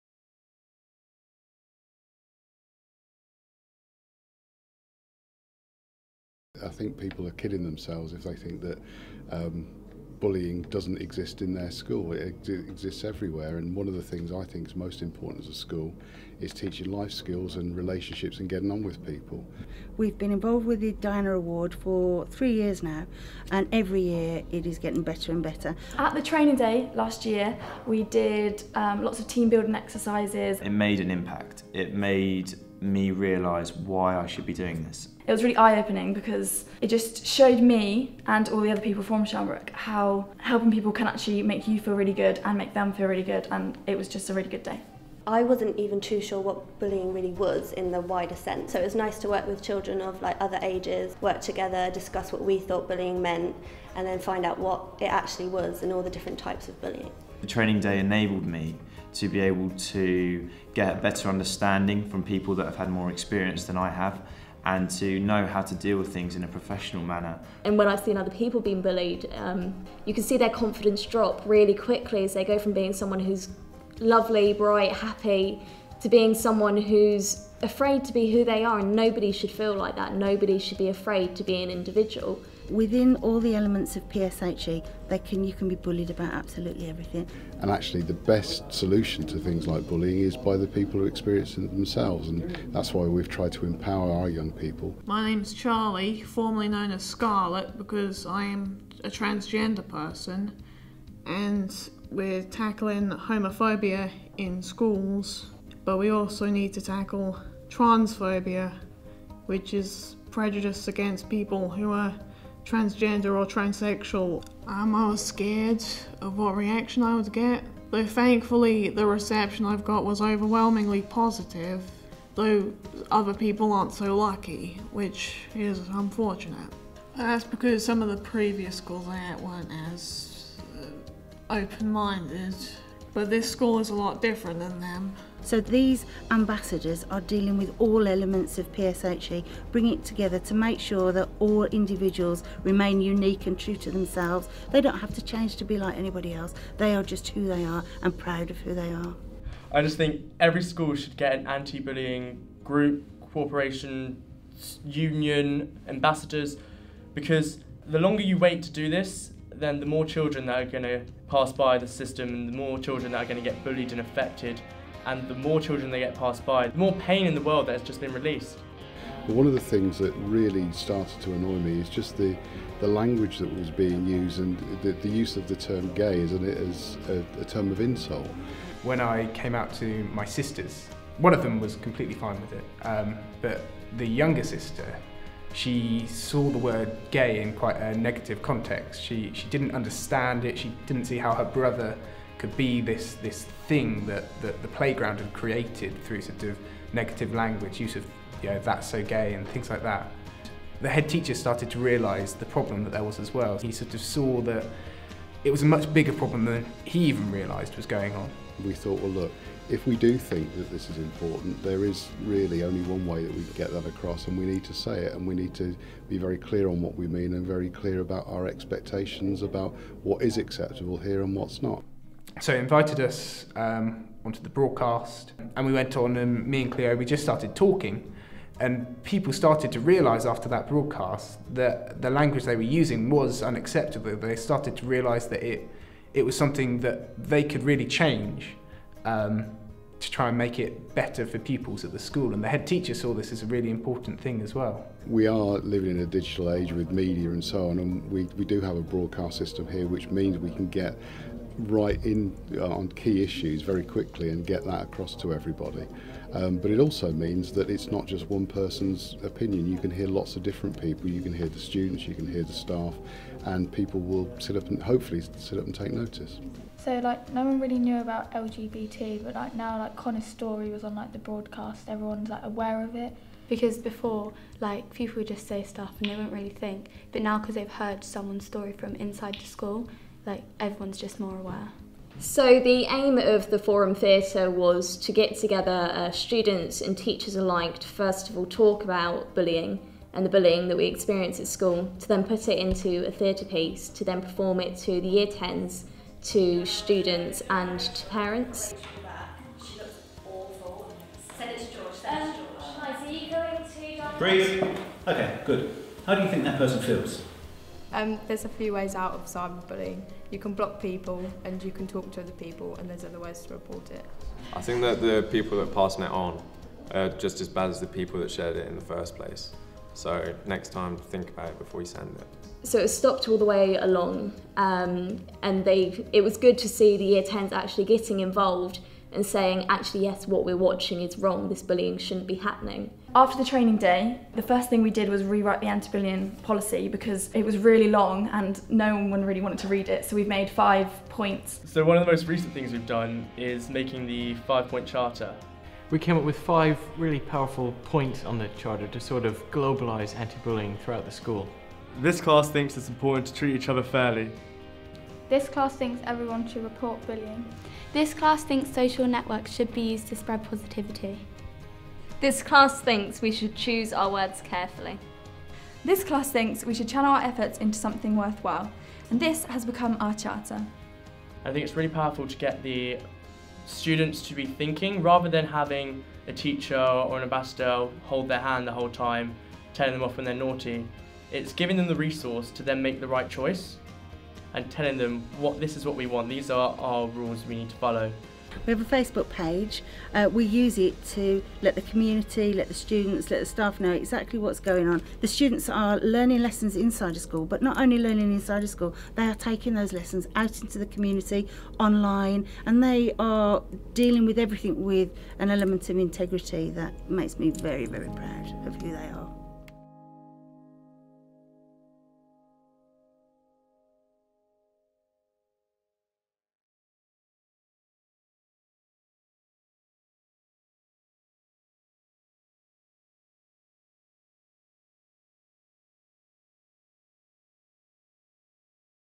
Anti-Bullying Ambassadors stand up to LGBT bullying at Sharnbrook Upper School
On Thursday 9th July 2015 we invited young people from across the UK to Facebook's Headquarters in London. Here we showcased and celebrated the anti-bullying work they have been doing in their schools and communities since they were trained to be Anti-Bullying Ambassadors

A big focus of Sharnbrook Upper School and Sixth Form’s campaign through the Anti-Bullying Ambassador programme has been challenging Homophobic, Biphobia and Transphobic bullying and championing difference.

For more information about the Diana Award’s Anti-Bullying Ambassadors programme, please go to: